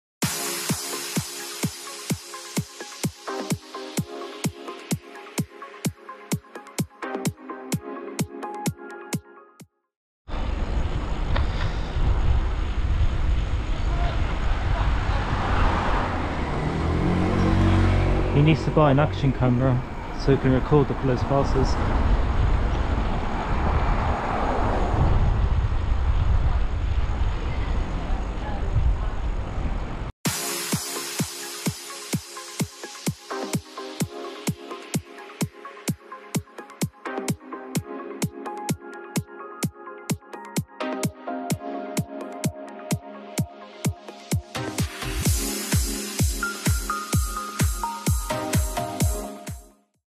Grandpa Almost Hits a Cyclist - LA19 HFV
Bad drivers never learn, therefore they need to be prosecuted. Perhaps, the realisation of being prosecuted will make them comply with the highway code.
The highway code hierarchy:

Pedestrians
Cyclists
Horses Riders
Motorcycles
Cars
Lorries
Have you had a bad 👎 or a great 👍 encounter with a driver? If so, feel free to rate the driver via the following
Video Details:
Action camera used: GoPro Hero 9
Editing software used: Adobe Premiere Pro
Offending Driver(s): LA19 HFV
Incident Location: Villiers Ave, Surbiton
Date of Incident: 29062024
Time of Incident: 18:14